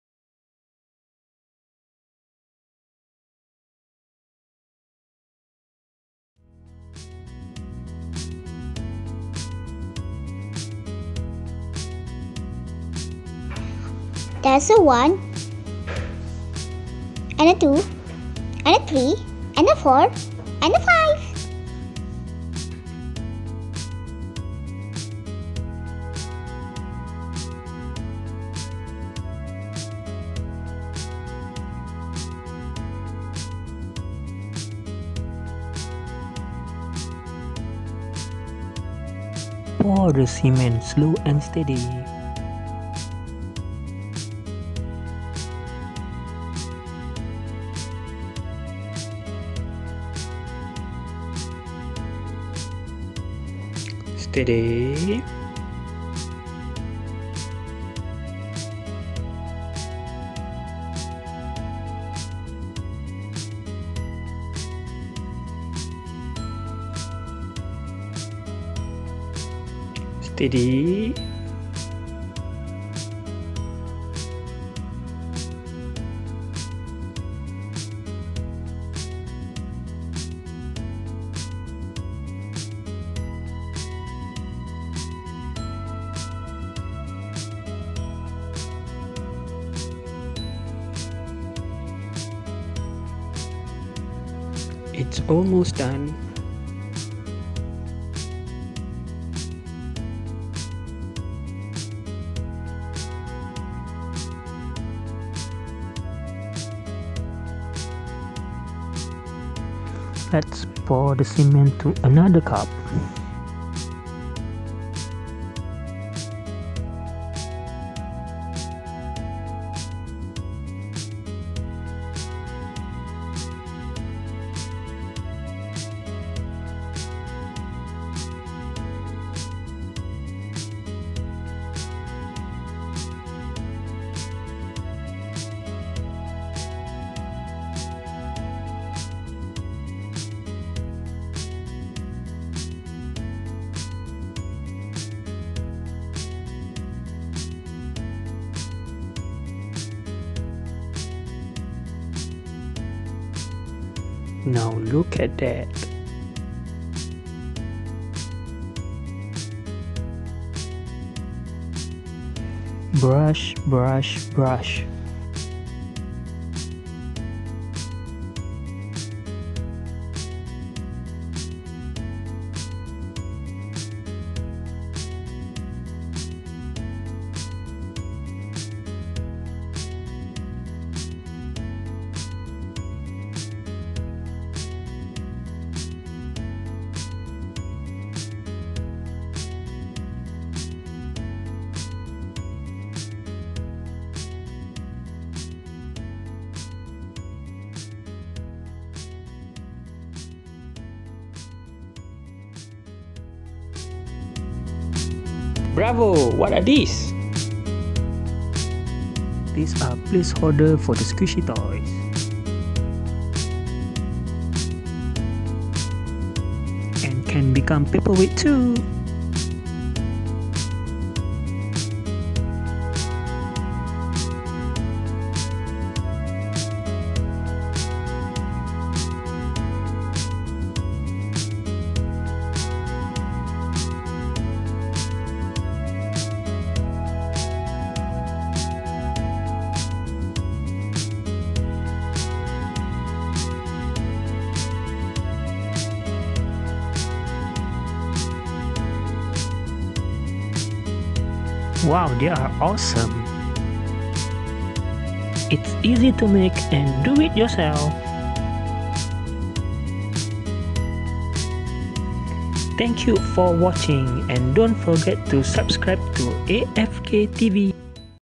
DIY Cement | How To Make A Placeholder Cement and Decorate Them with BT21 Squishy Toys
Let's do it yourself (DIY) cement and make placeholder and decorate them with cute BT21 squishy toys.

It's easy and simple to do.

1. Use empty paper cups
2. Stir cements with water
3. Pour the mixed cements in the paper cups.
4. And decorate them.

Amazing!!!

We hope you enjoy watching the video.

Find us at IG @afktvhavefun